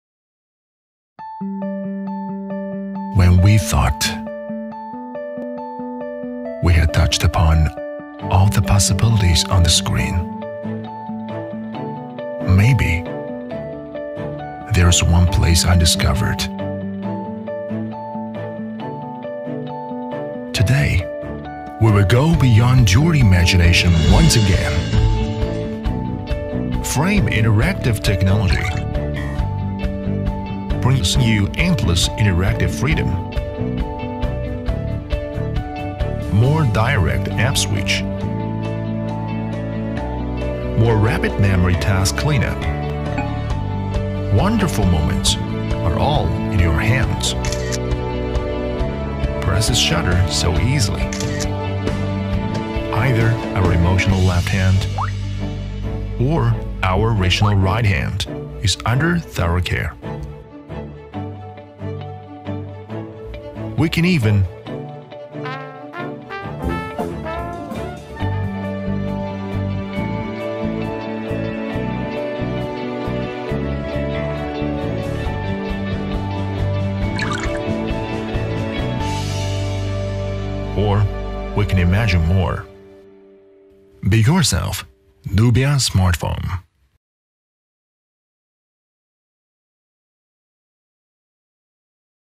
Nubia upcoming bezel less phone
Nubia is launching a bezel less smartphone with interactive sliding system
 Awesome phone watch and wait for its coming.